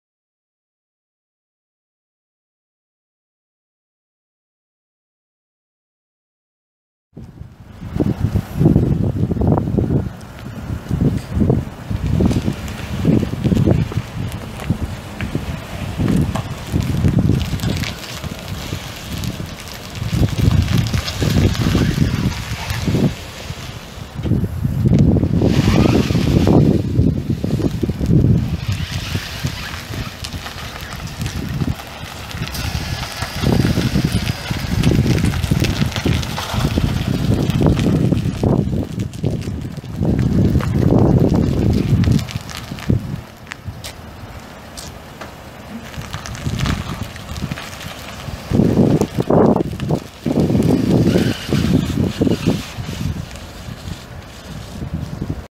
2011 Chevrolet Tahoe Police SUV for sale | no-reserve Internet auction April 26, 2017
2011 Chevrolet Tahoe Police SUV
70,291 miles on odometer
5.3L V8 OHV 16V FFV CNG engine
Automatic transmission
Tilt
Cruise
AM/FM/CD
Power windows, locks, mirrors, and seats
Cloth interior
Spotlight
(3) 12V outlets
Sound off signal box
Rifle/shotgun rack
Digital Ally rearview mirror
Camera and office microphone
Serial LA10BF52
Pinicle light bar
Go Rhino cage
Plastic prisoner seat
Running boards
Go Rhino grill guard
D and R storage box with sliding drawer
Receiver hitch
Trailer wiring
265/70R17 tires
Emergency/Police equipment will be removed if purchased by a civilian
Sale# 1730KP
Unit# 9
Oklahoma title
1GNSK2E03BR241774